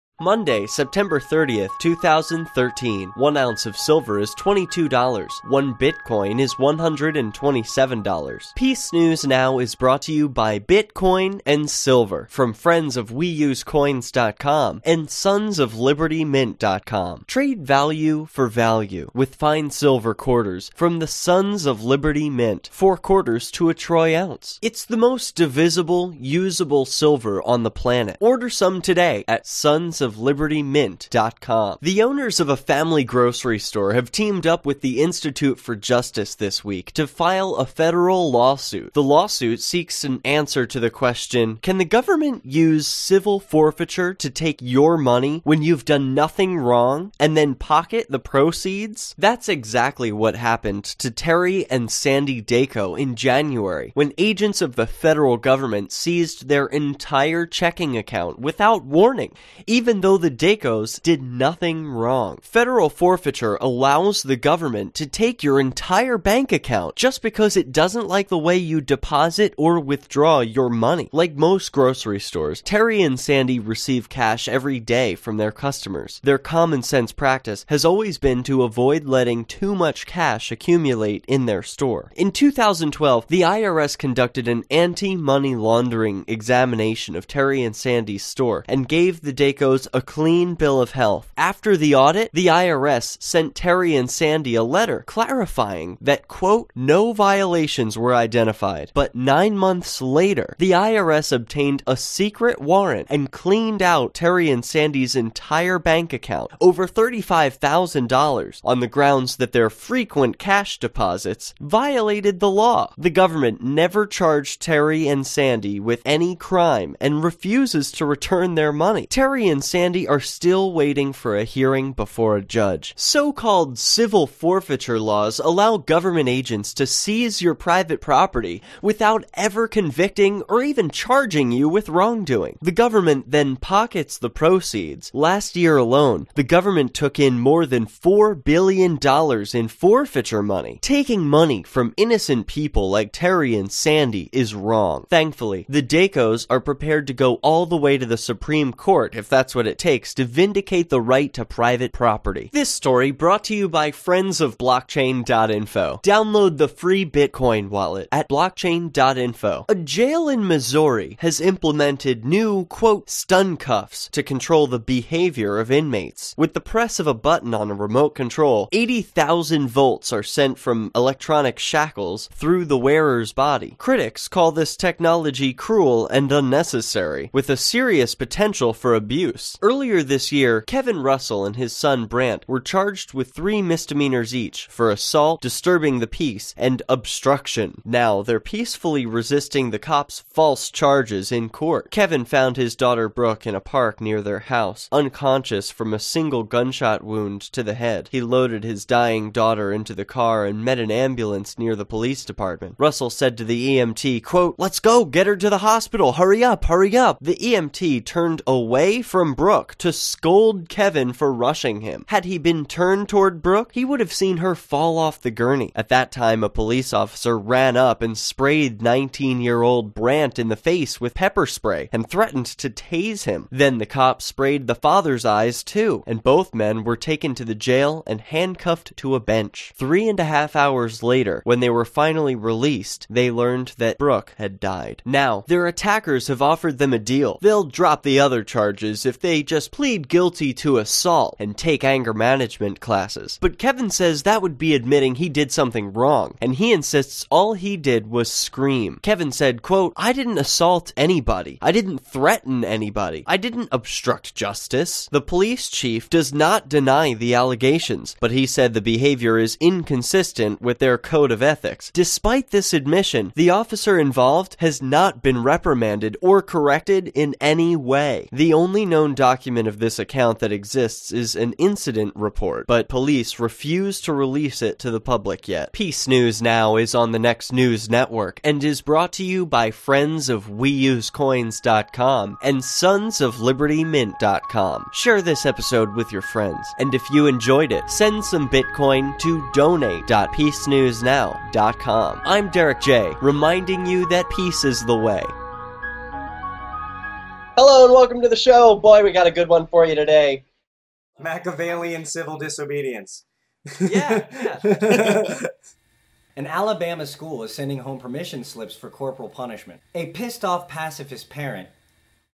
Government Agents are Officially Gangsters
Grocery Store has $35,000 seized, no charges filed